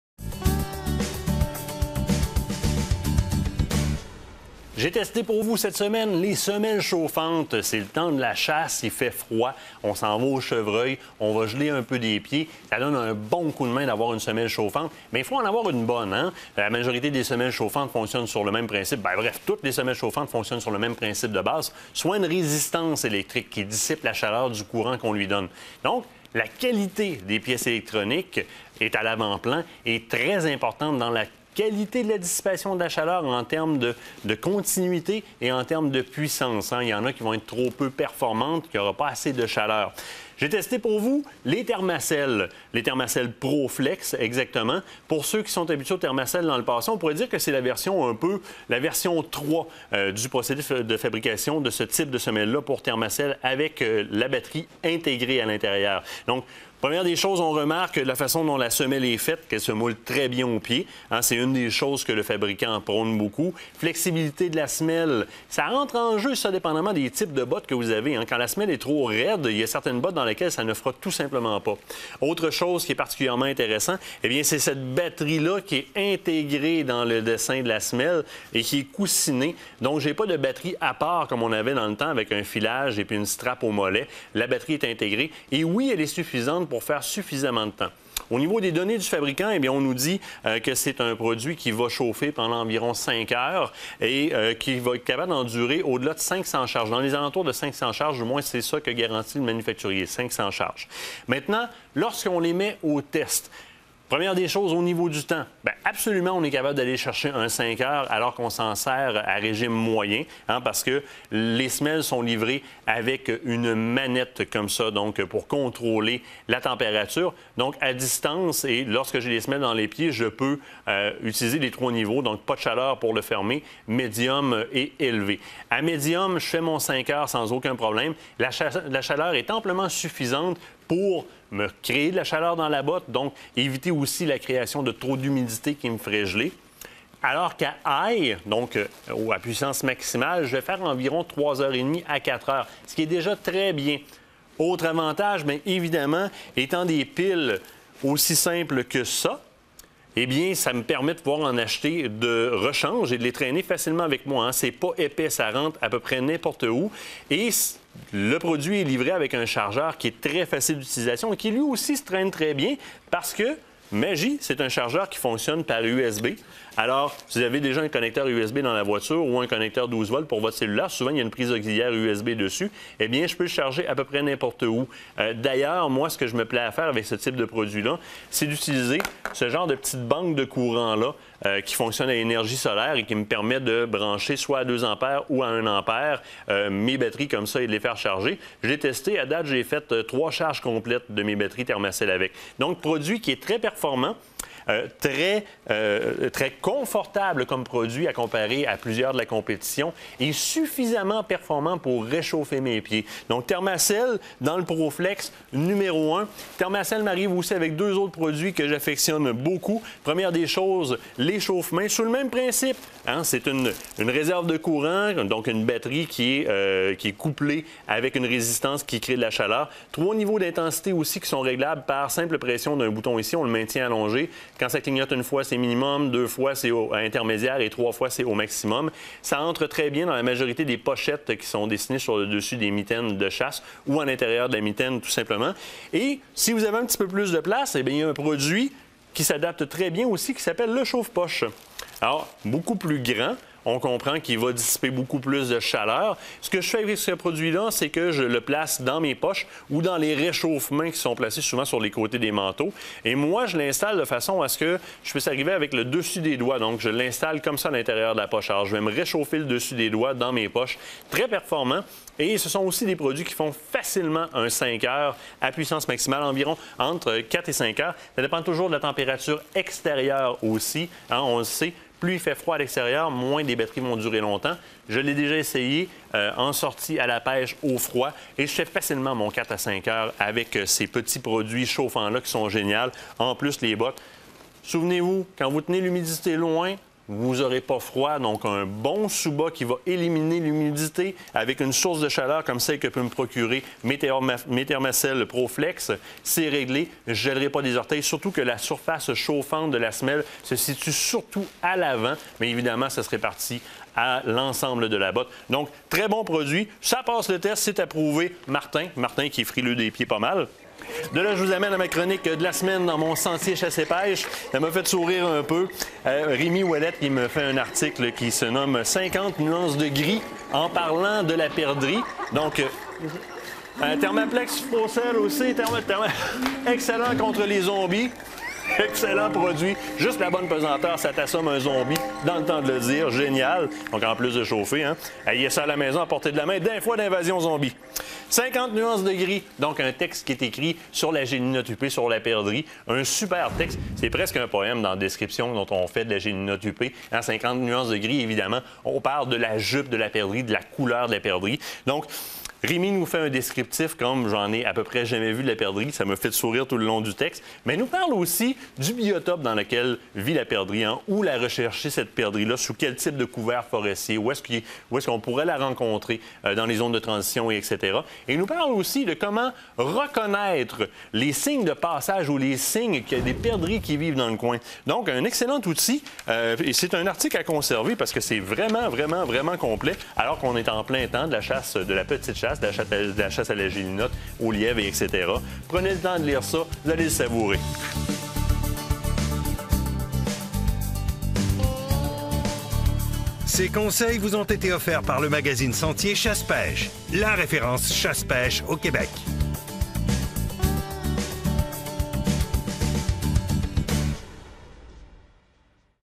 Les produits chauffants ThermaCELL
Avec Martin Bourget de Loisirs, Chasse et Pêche à TéléMag.